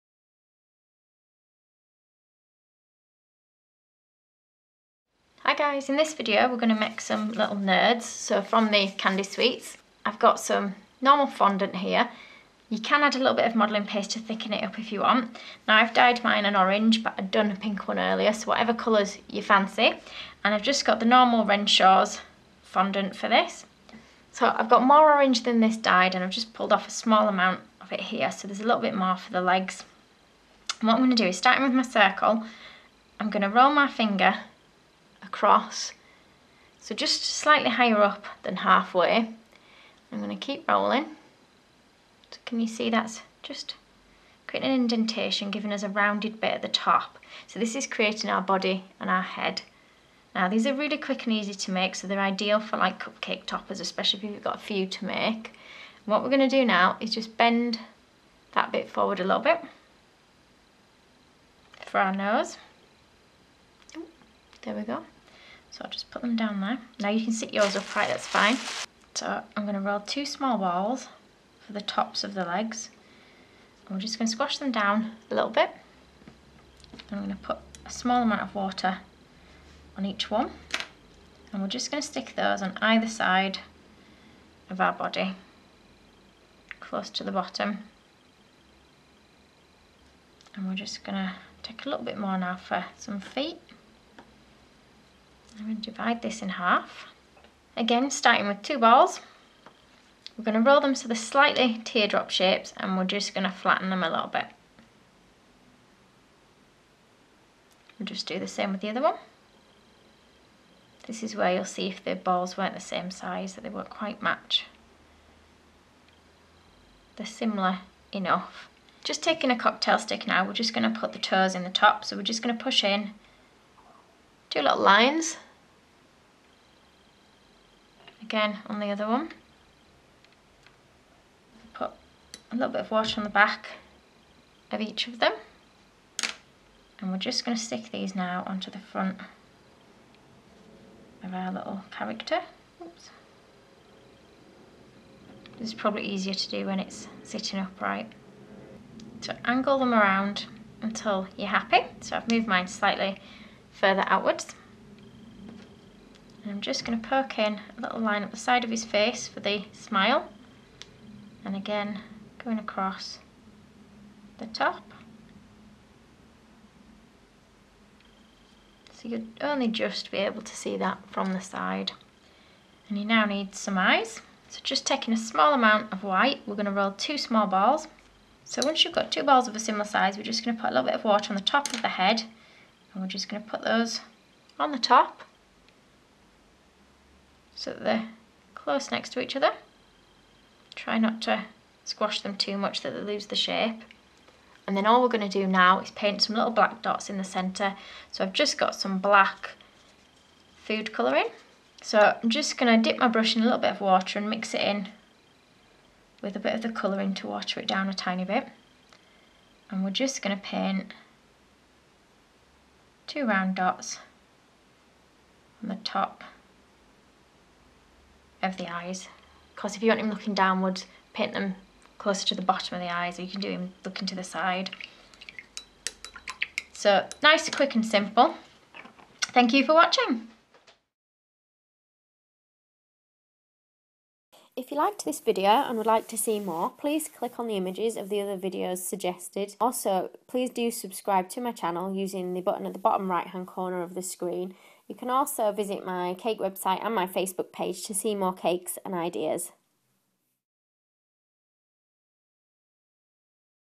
Fondant Nerds cake topper models: Wonka Nerds
Wonka Nerds cake topper model using fondant.
In this cake modelling tutorial I show you how to make some Wonka Nerds cake toppers using fondant, these are fairly small and could even be used as cupcake toppers! I used fondant for this but you could also use flower and modelling paste, gum paste, mexican paste or fimo / polymer clay / sculpey if you do not want your Nerds model to be edible :)

My premium tutorial, sugar craft rose making course, with exclusive discount already applied